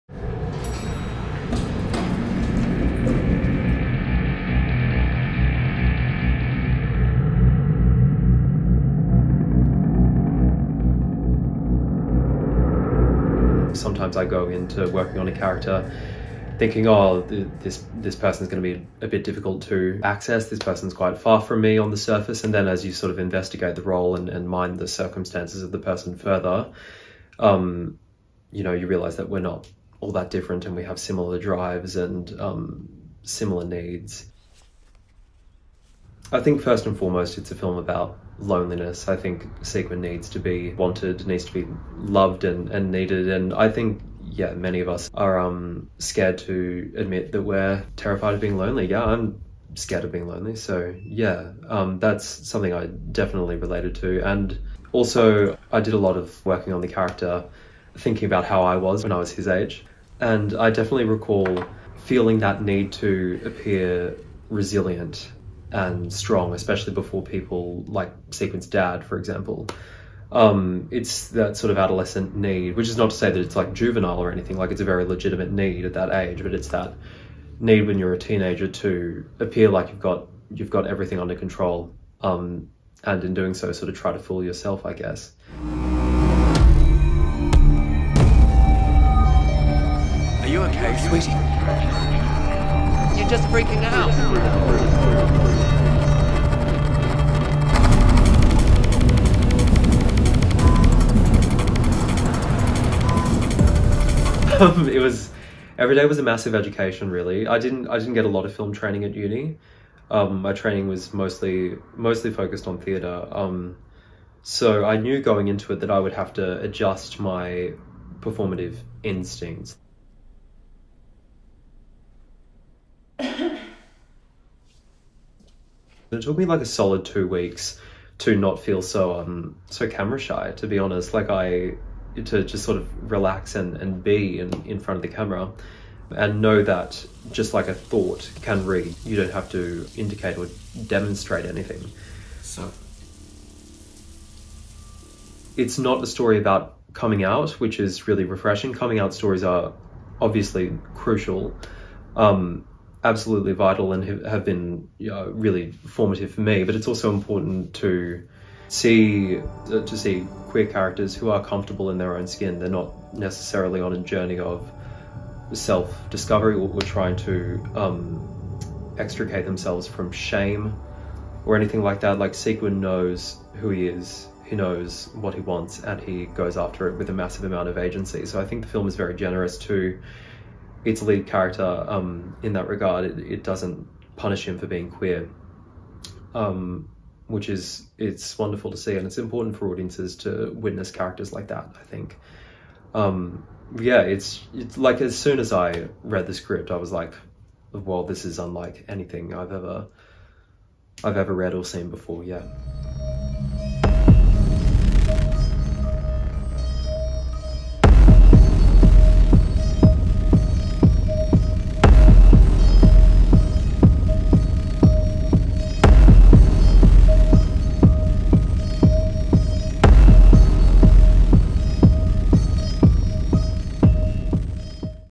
SEQUIN IN A BLUE ROOM - Interview - Conor Leach
US, Canada & Scandinavia and Elsewhere from May 17

SEQUIN IN A BLUE ROOM is the story of Sequin, who is exploring his burgeoning sexuality through an obsession with a meet-up app. That is until he finds his way into The Blue Room - where a whole new, alluring world unfolds before him. There, Sequin connects with a captivating stranger, but they are separated suddenly. Utterly fixated on this man, Sequin sets off on an exhilarating and dangerous mission to track him down.

Cowritten by Jory Anast and Samuel Van Grinsven, Sequin in a Blue Room is a highly-accomplished queer coming-of-age tale and a breath of fresh air from the Australian independent film scene.